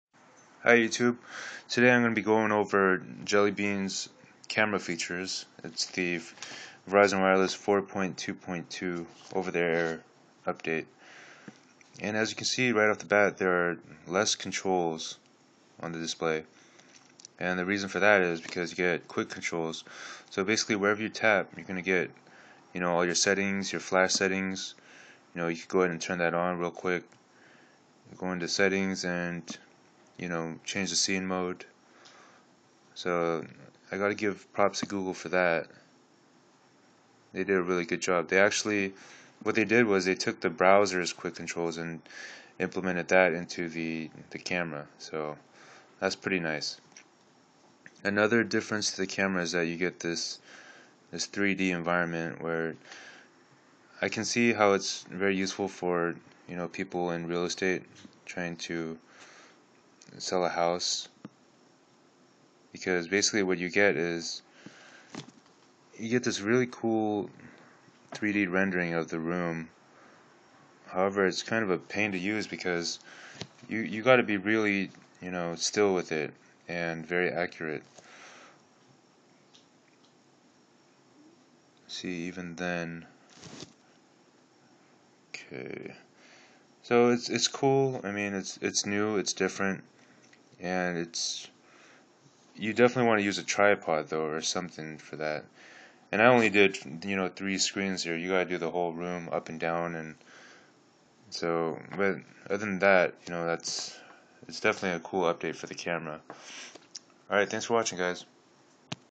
VZW JellyBean 4.2.2 OTA Update: Camera Features
Verizon Wireless 4.2.2 ota update.  This video portrays the new camera settings and interface for the samsung galaxy nexus